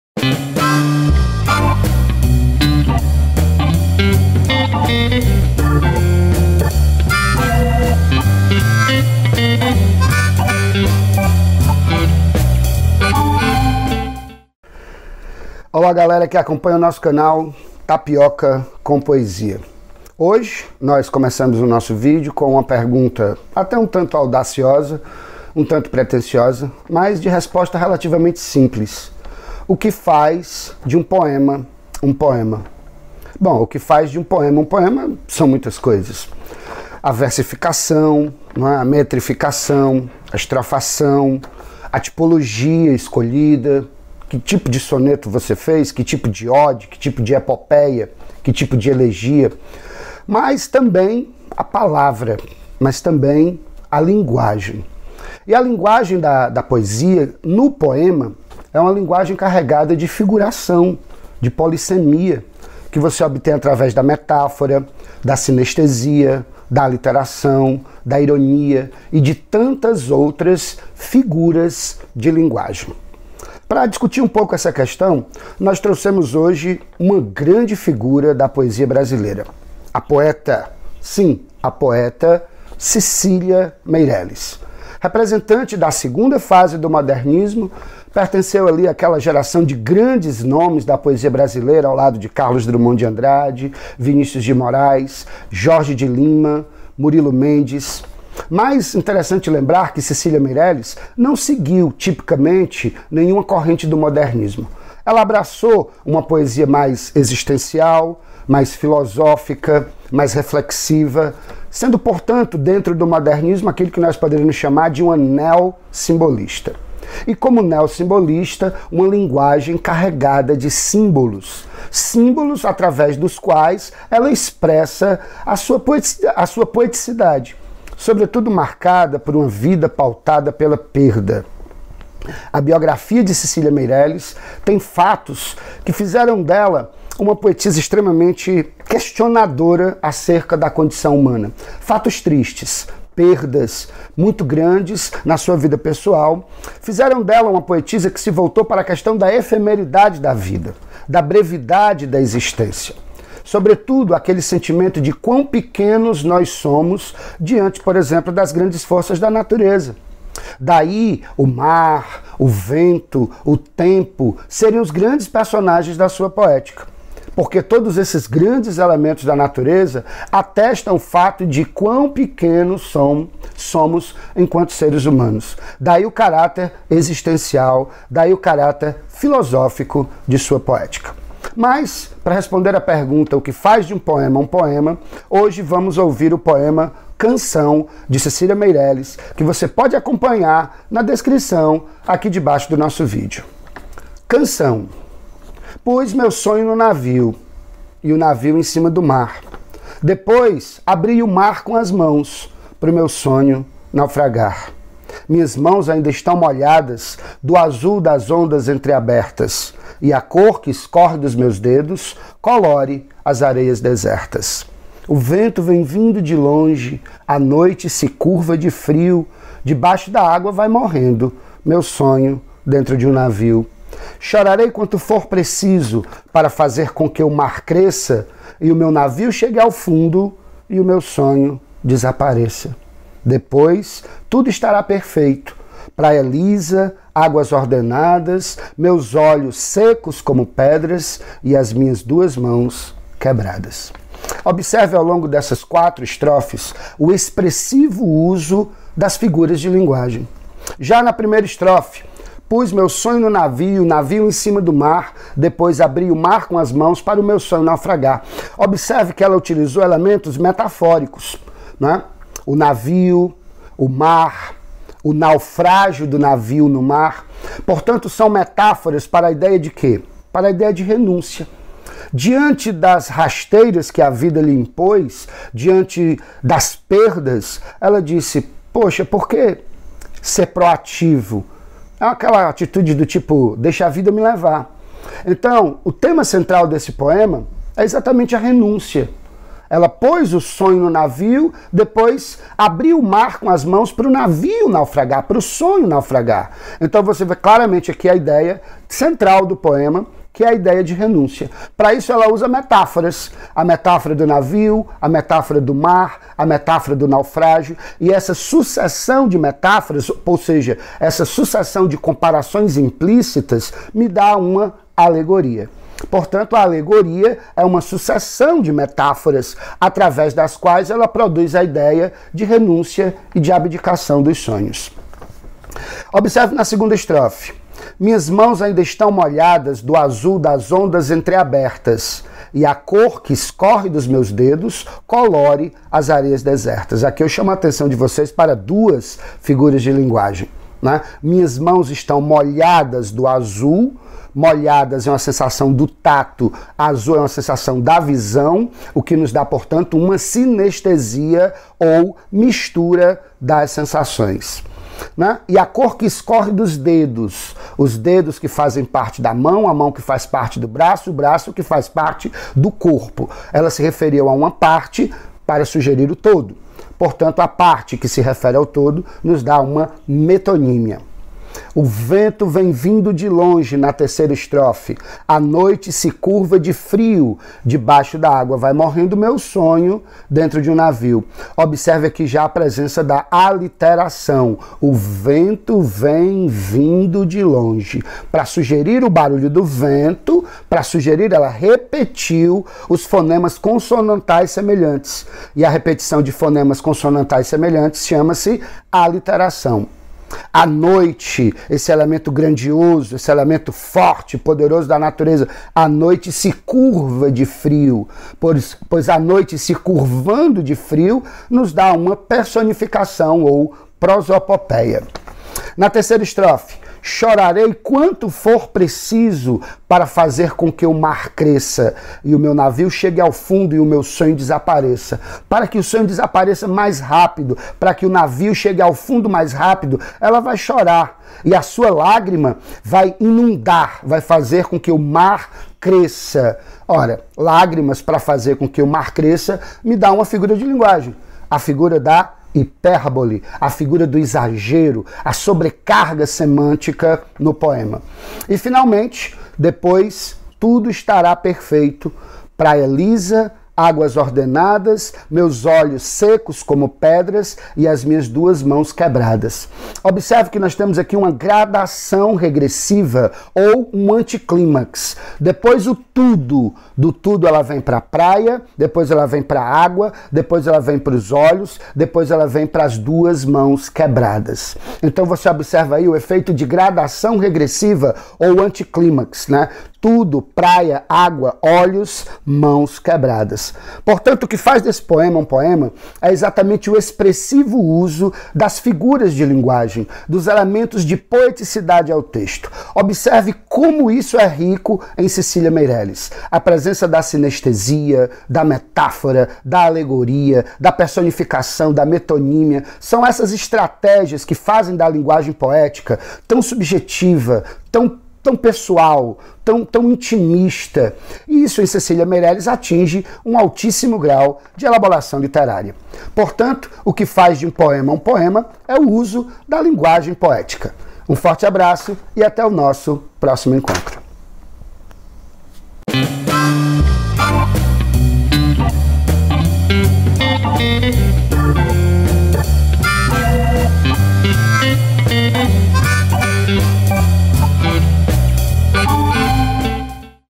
O que faz de um poema um poema?Cecília Meireles nos ajuda a responder
Análise das figuras de linguagem no poema Canção de Cecília Meireles.acompanhe a leitura

Canção

Pus o meu sonho num navio
e o navio em cima do mar;
- depois, abri o mar com as mãos,
para o meu sonho naufragar.

Minhas mãos ainda estão molhadas
do azul das ondas entreabertas,
e a cor que escorre de meus dedos
colore as areias desertas.

O vento vem vindo de longe,
a noite se curva de frio;
debaixo da água vai morrendo
meu sonho, dentro de um navio...

Chorarei quanto for preciso,
para fazer com que o mar cresça,
e o meu navio chegue ao fundo
e o meu sonho desapareça.

Depois, tudo estará perfeito;
praia lisa, águas ordenadas,
meus olhos secos como pedras
e as minhas duas mãos quebradas.

Cecília Meireles MEIRELES, C. Antologia Poética. Rio de Janeiro: Editora Nova Fronteira, 2001.